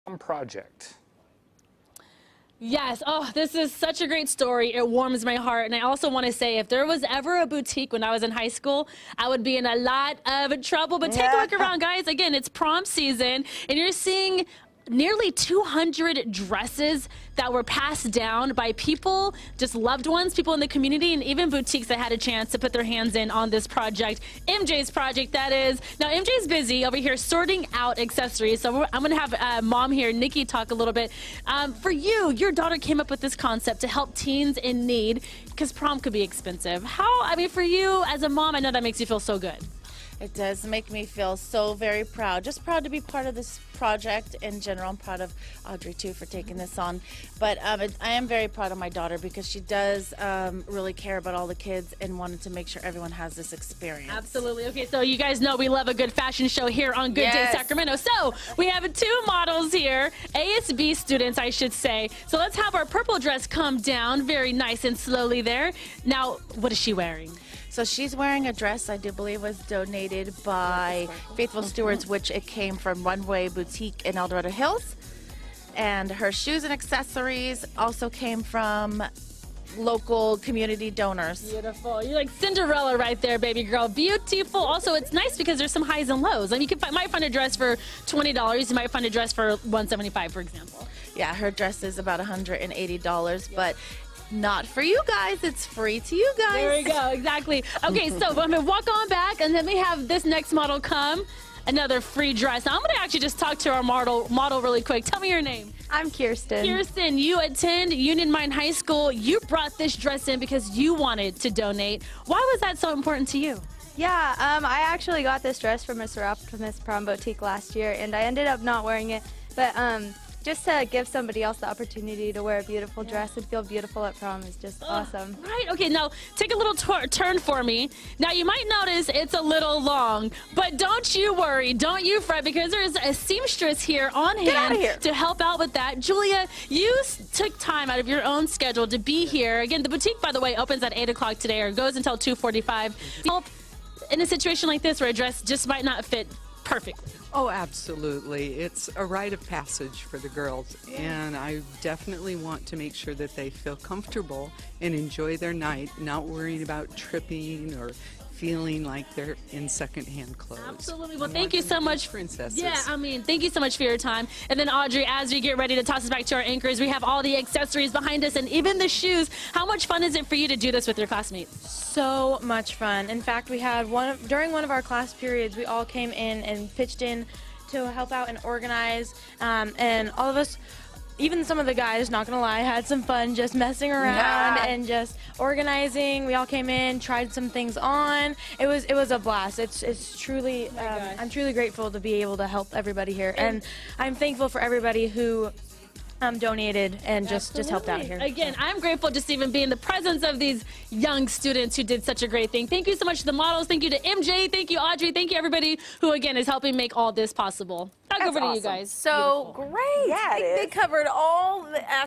MJ's Project Pt. 2
Ashley Williams has more on this project to help kids get to prom for a low cost.